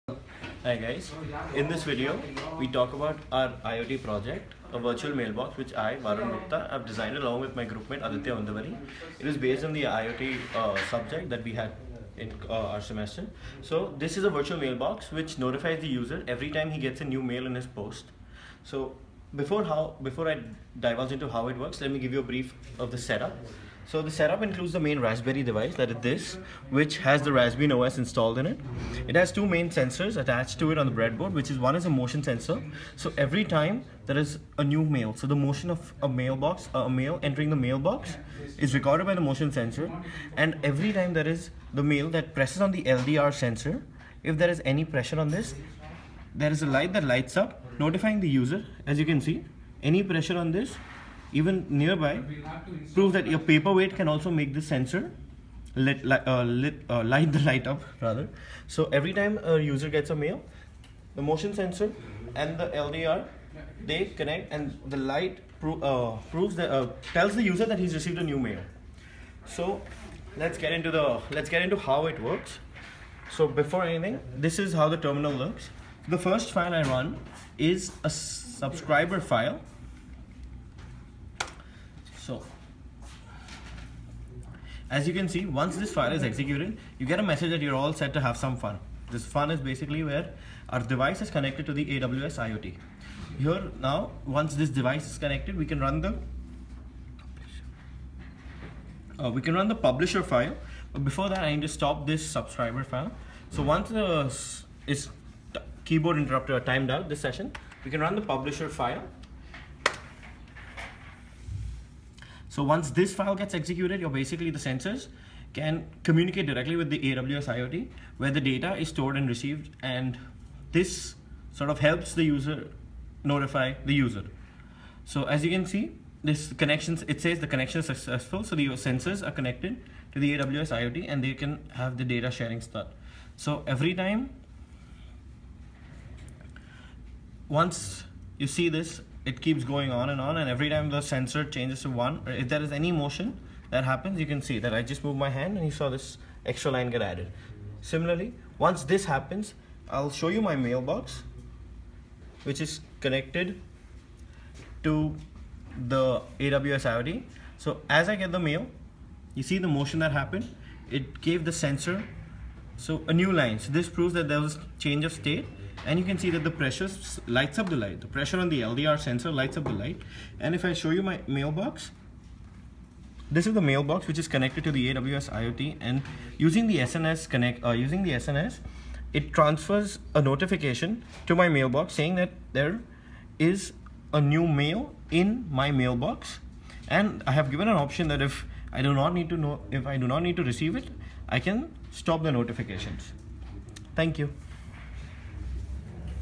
IoT Project Demo- Virtual Mailbox- Rasberry Pi
Video explains the working of the Virtual Mailbox and it's setup. Also explains how AWS SNS service works.